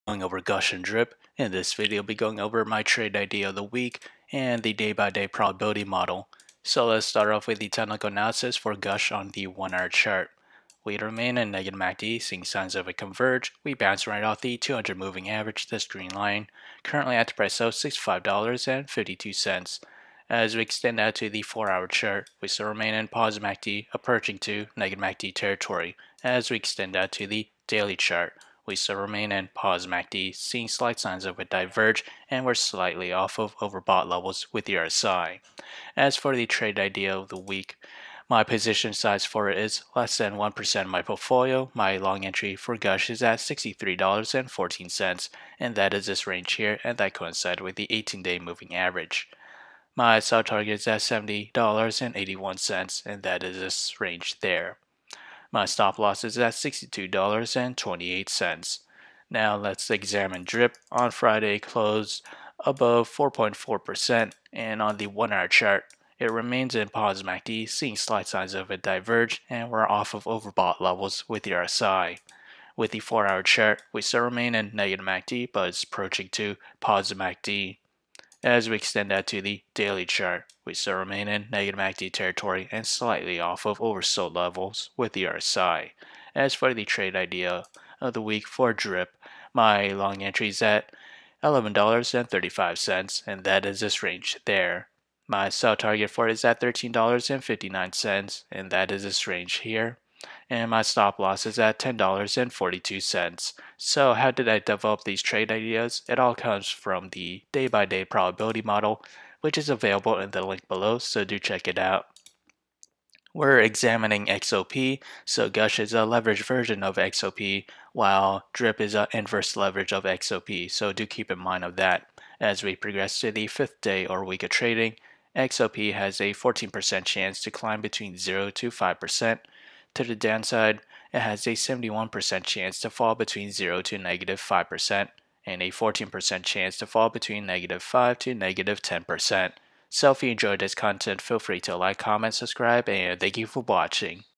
GUSH & DRIP (WTI Crude Oil) for February 27 2021 Technical Analysis, Forecast, and Trade Ideas
United States Oil Fund (USO) tracks the movement of Crude Oil Futures. It is an investment tool to reflect the daily changes in spot price of oil. It provides investors access to futures markets in the form of etf. The contracts are primarily for light, sweet crude oil or specifically the West Texas intermediate Light sweet Crude Oil. Other exposures to oil include SCO for bearish movement and UCO for bullish movement. These etfs are 2x the movement of USO. Other exposures include GASL, DRIP, GUSH, GASX, which are 3x movement of oil and gas exploration companies (XOP & XLE).